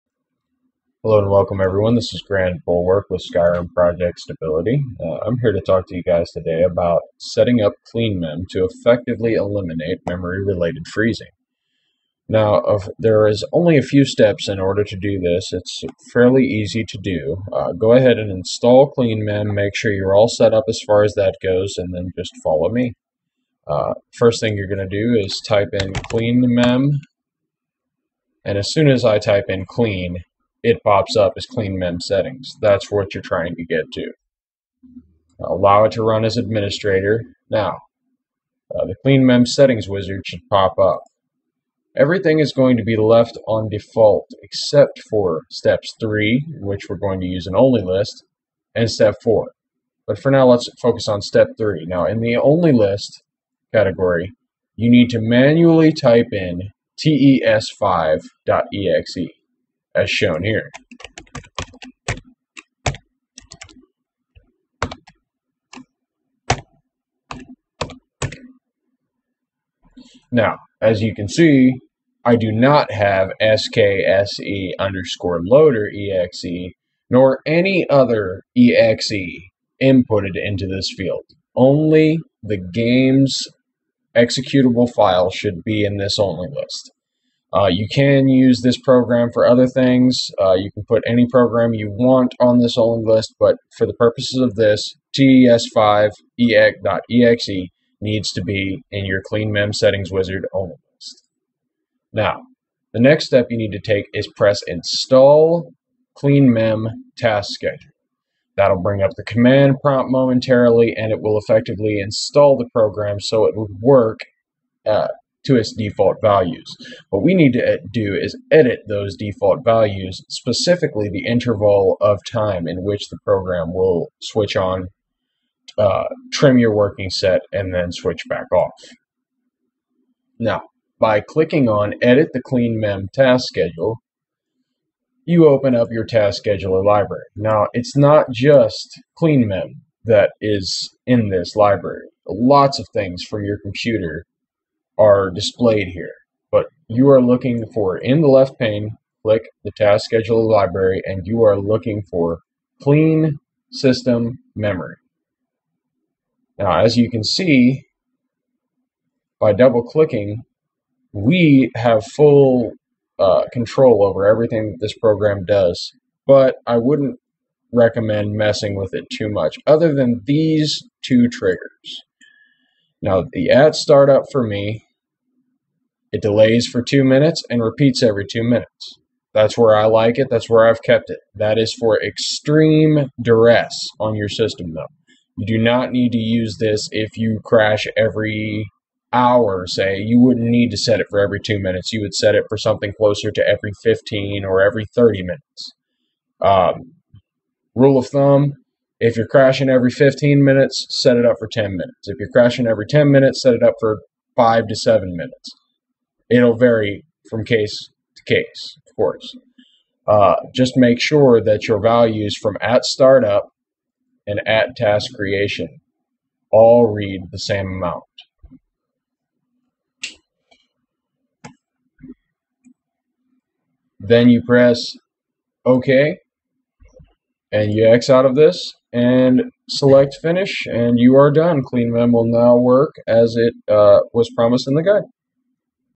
Cleanmem Set Up
A guide to detting up Cleanmem for use with The Elder Scrolls 5, Skyrim.